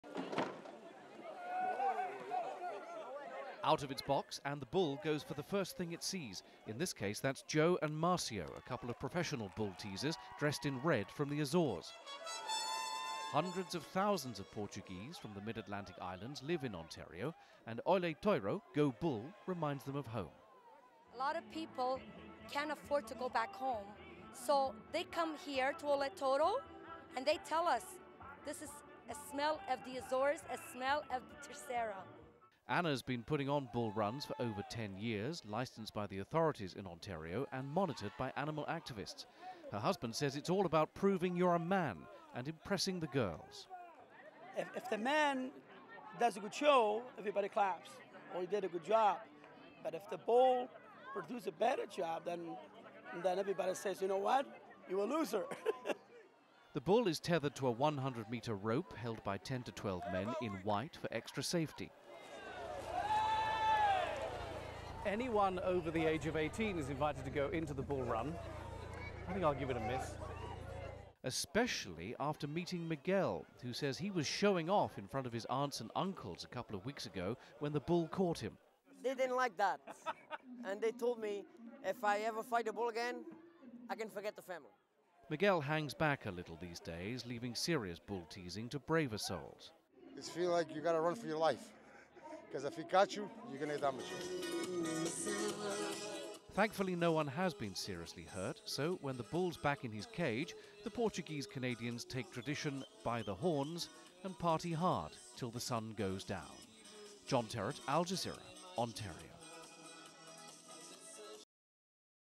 BULL Run.mov
On a farm north of Toronto, Portuguese Canadians celebrate an age-old tradition brought from their homeland in the Azores.  This sport, the running of the bulls, done without killing the animals, helps these families feel closer to their past and their relatives abroad.